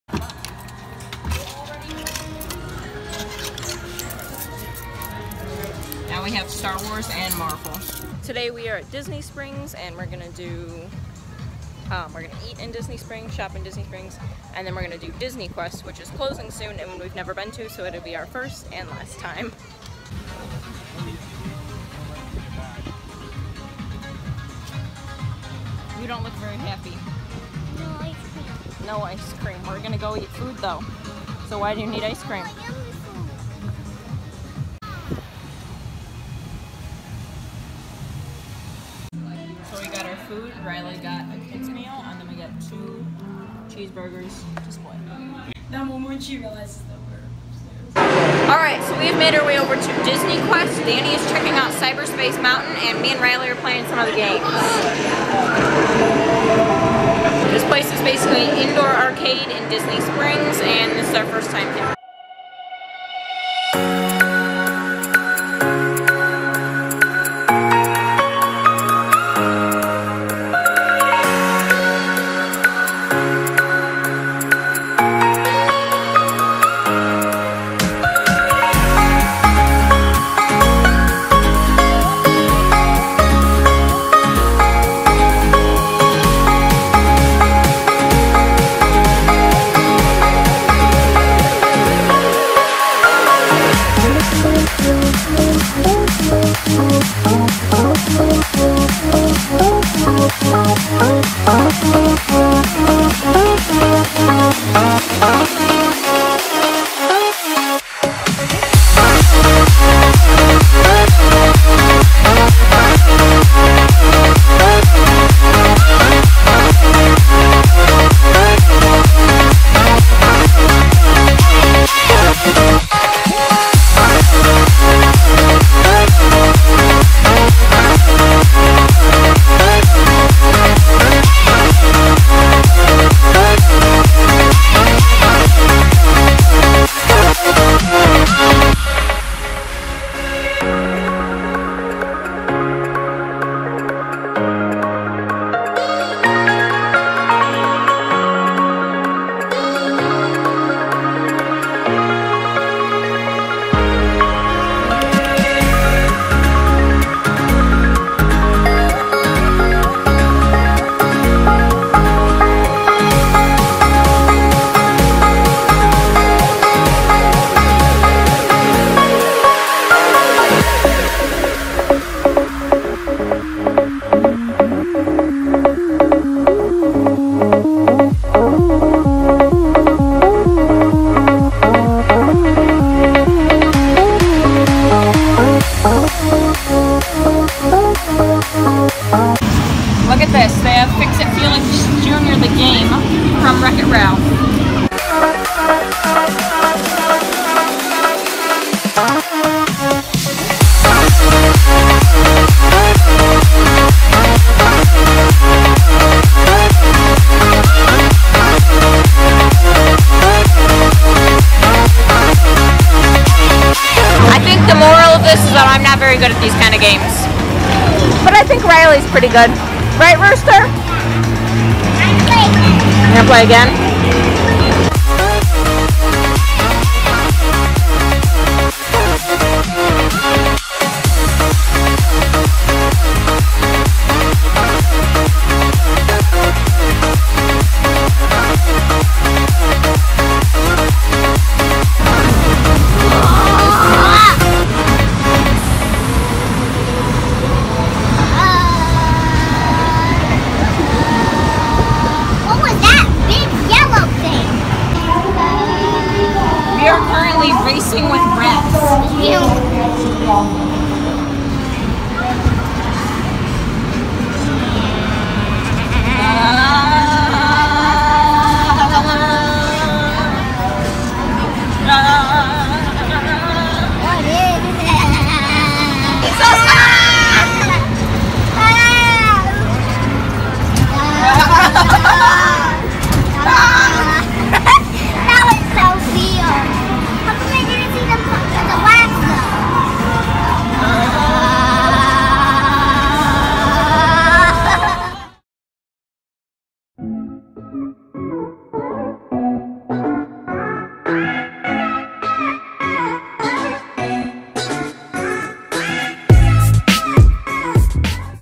DISNEY SPRINGS🛍
On our 5th day at Disney, we head over to Disney Springs! This is kind of a short vlog, but still a good one! We do some shopping, which I didn't record very much of. Then, we enjoy burgers from D-Luxe Burgers, which were SO GOOD! After that, we go to Disney Quest, which is a huge indoor arcade that closed down on July 1st, 2017. We made it over there just before it closed, so we got to see it for the first time, as well as the last!

Hey! We are always open to suggestions, so if you would like to see a certain kind of video, leave it down below!